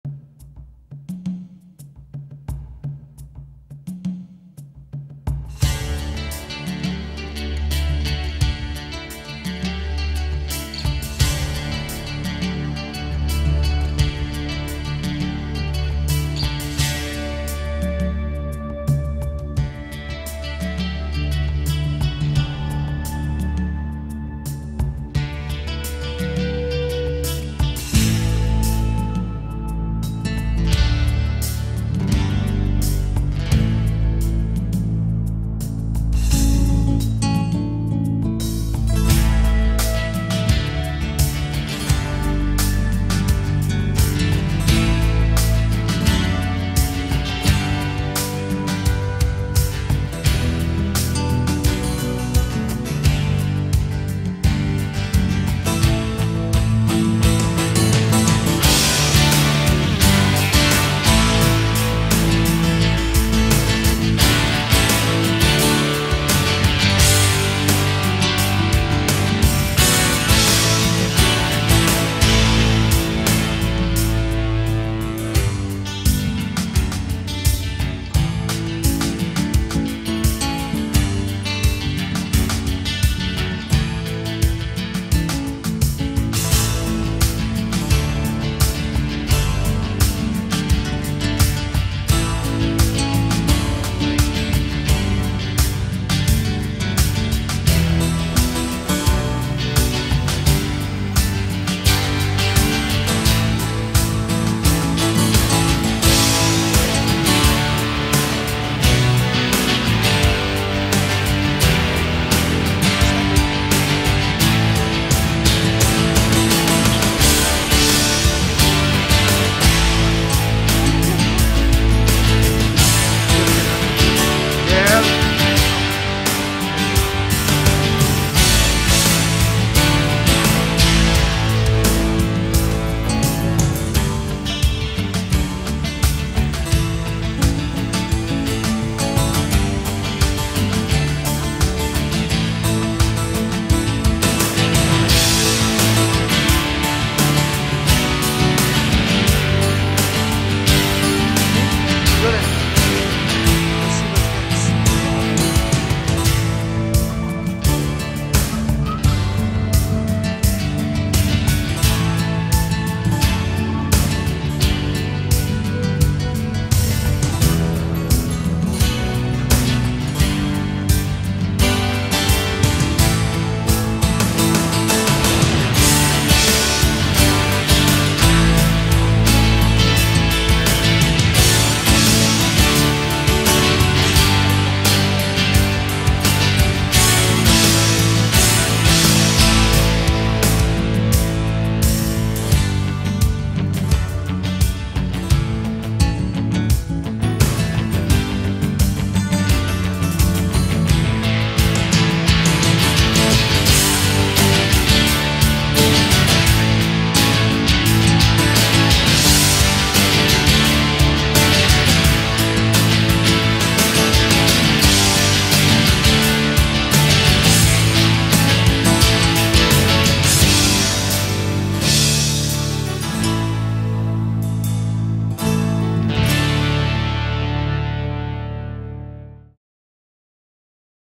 Westgate Flamingo Bay Resort Las Vegas Vacation 2023
Drone Footage (Recreational) and iPhone videos.